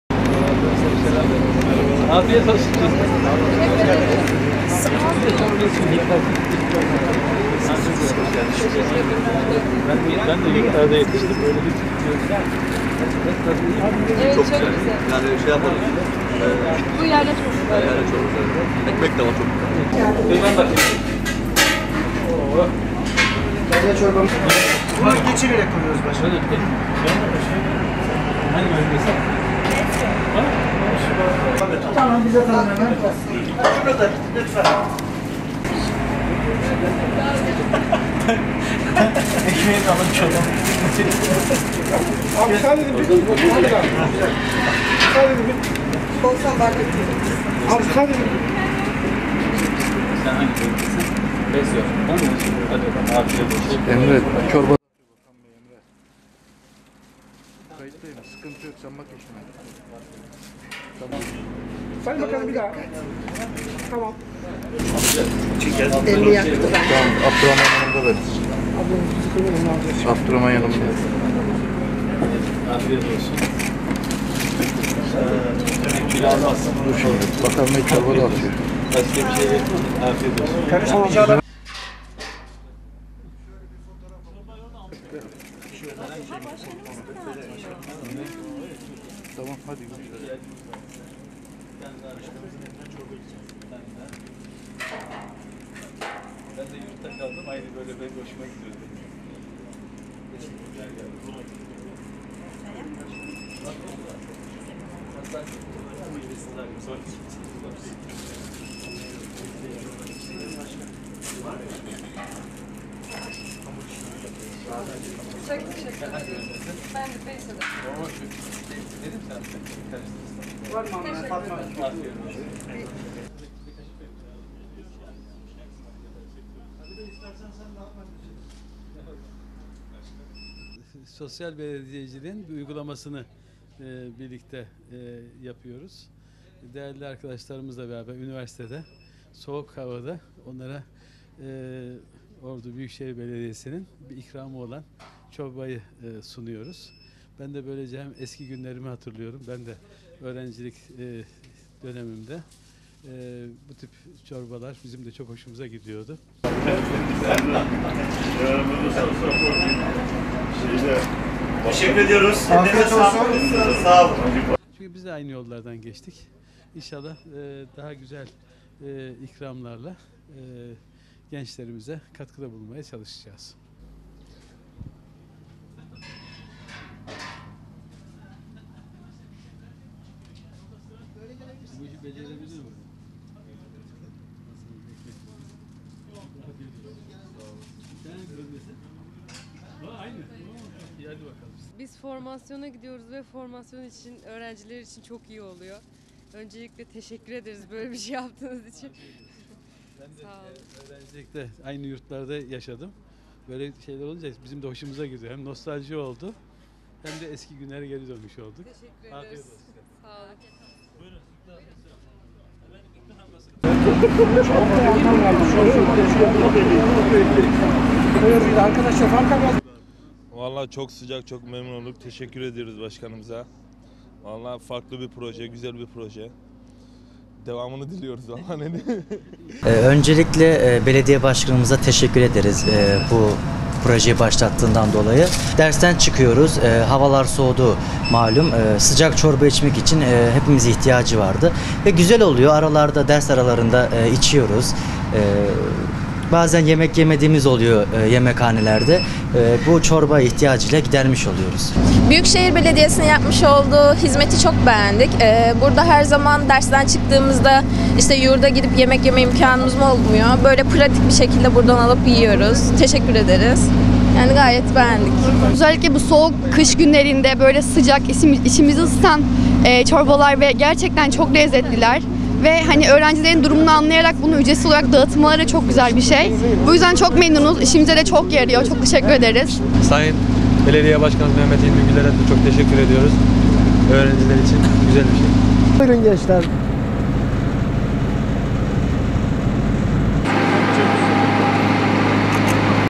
Büyükeşehirden Sosyal Belediyecilik Örneği
Ordu Büyükşehir Belediye Başkanı Dr. Mehmet Hilmi Güler, Ordu Üniversitesinde “Ders Arası Çorba Molası” uygulamasıyla öğrencilere çorba ikramında bulundu.